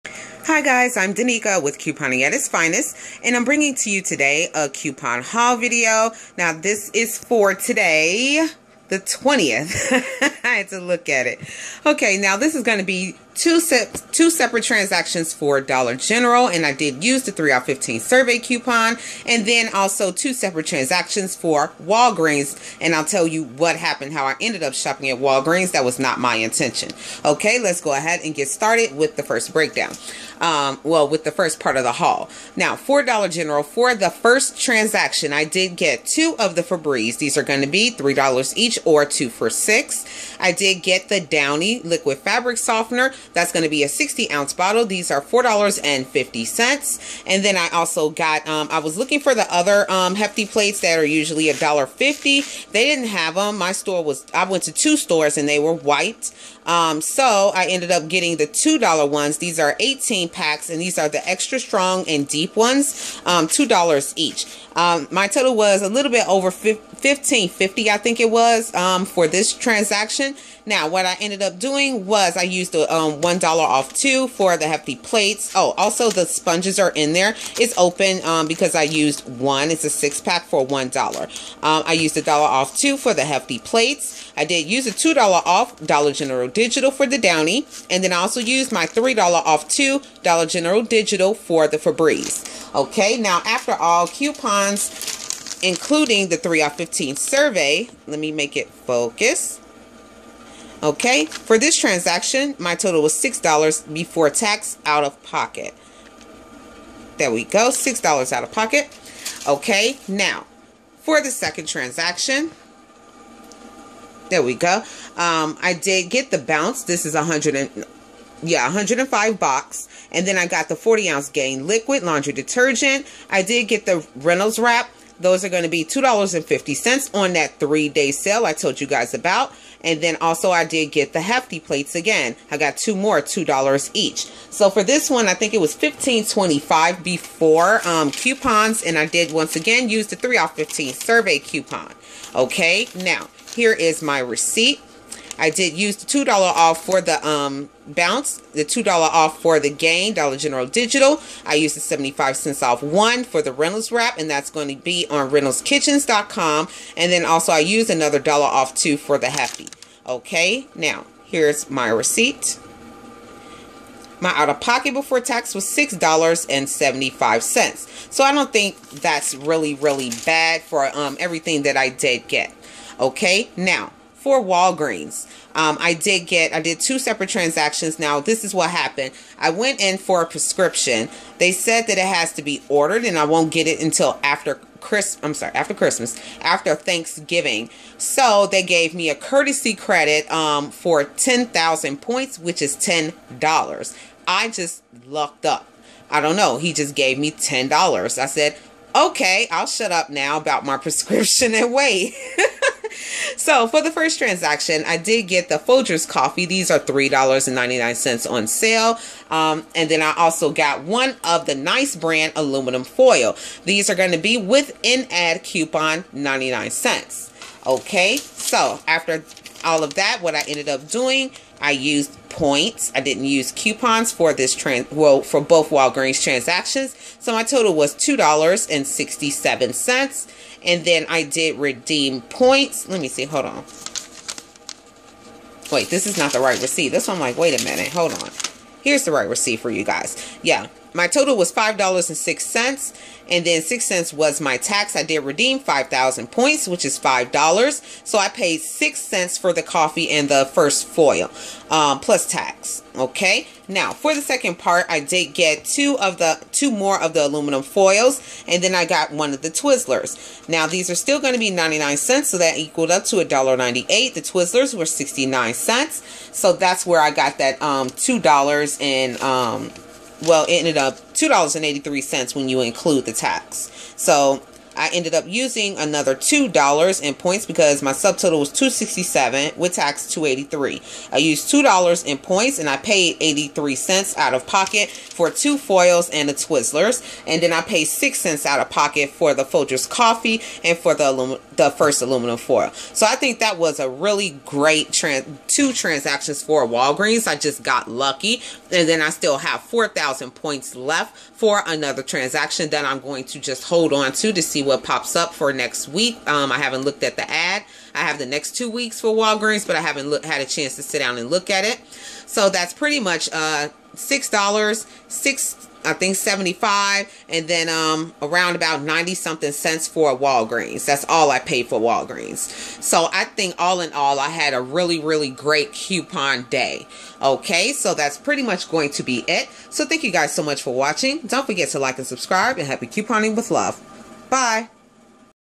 Coupon Haul 11/20 $6 oop Dollar General and Walgreens !!!
Hi guys. I did 2 transactions for Dollar General with the $3/$15 survey coupon and 2 transactions for Walgreens paying with points. Happy couponing with love and enjoy.